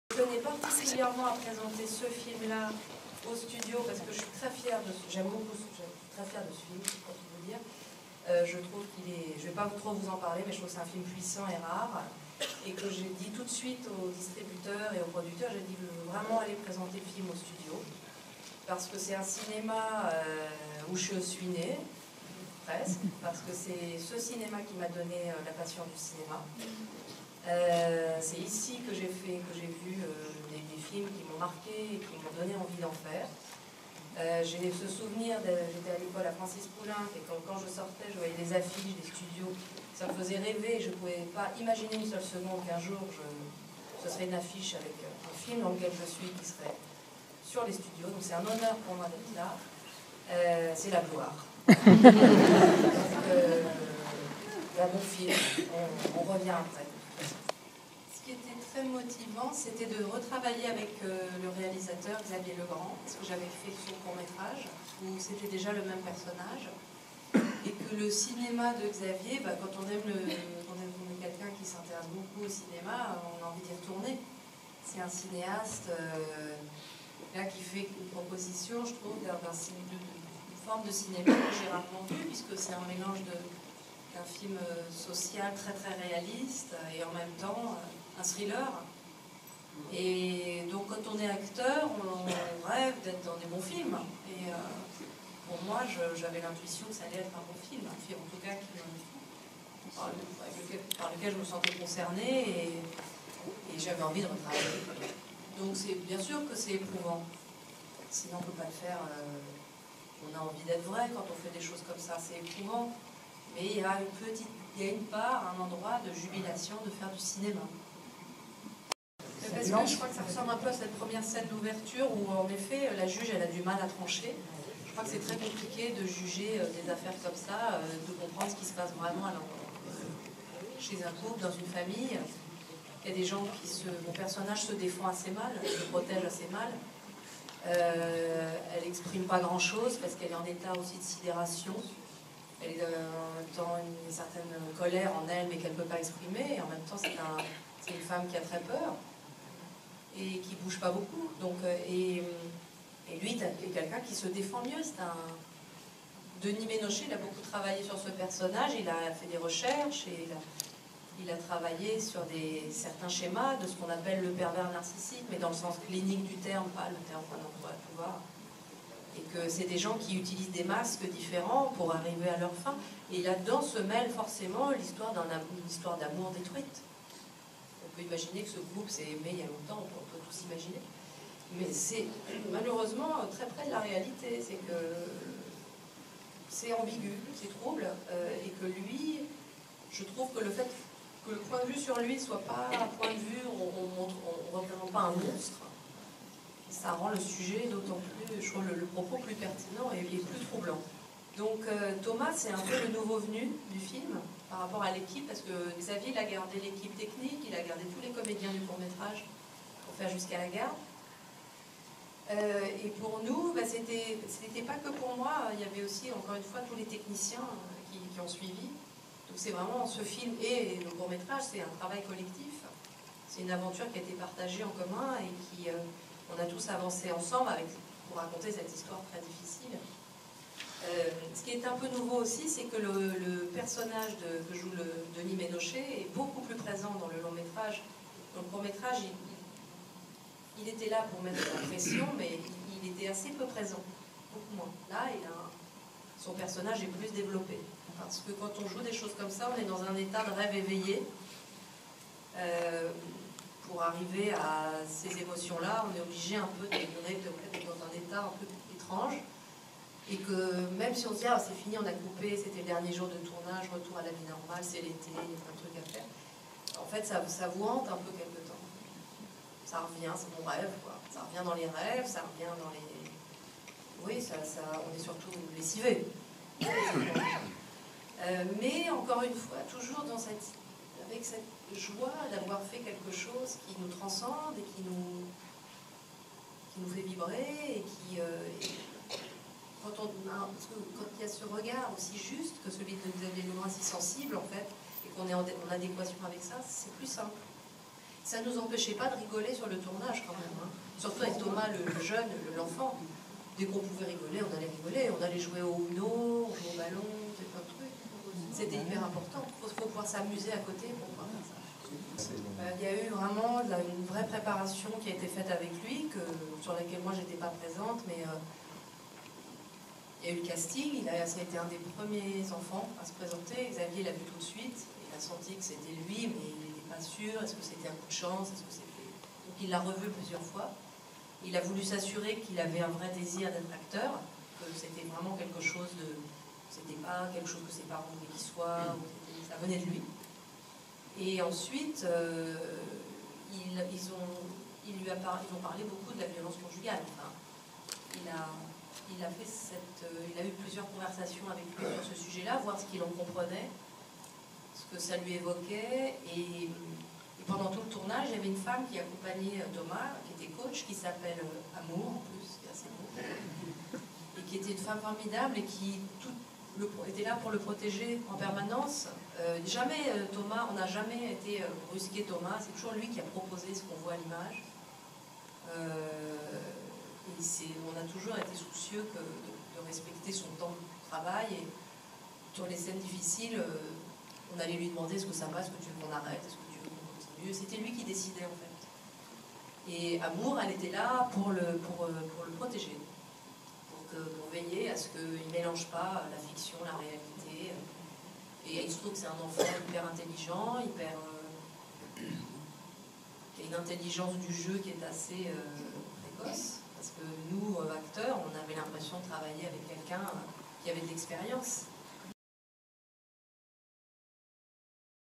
Rencontre Léa Drucker
Mercredi 31 janvier, Léa Drucker est venue présenter, aux cinémas Studio de Tours, le remarquable premier long métrage de Xavier Legrand intitulé "Jusqu'à la garde" dont elle est l'actrice principale aux côtés de Denis Ménochet...